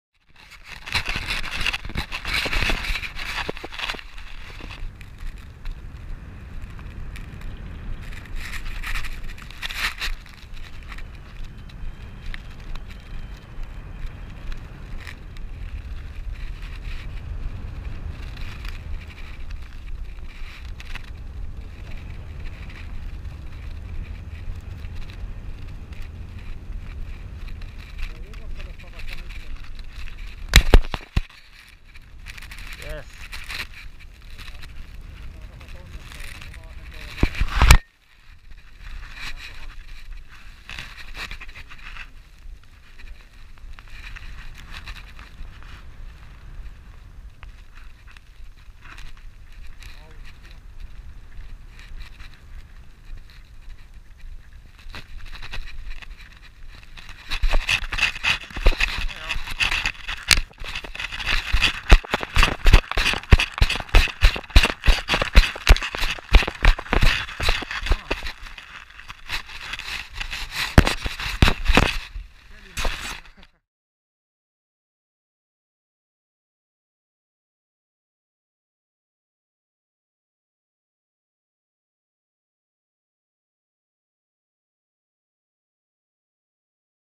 Varjoliitoa Jamilla
Lyhyt katsaus Jämin taivaalle varjoliitimen kyydissä.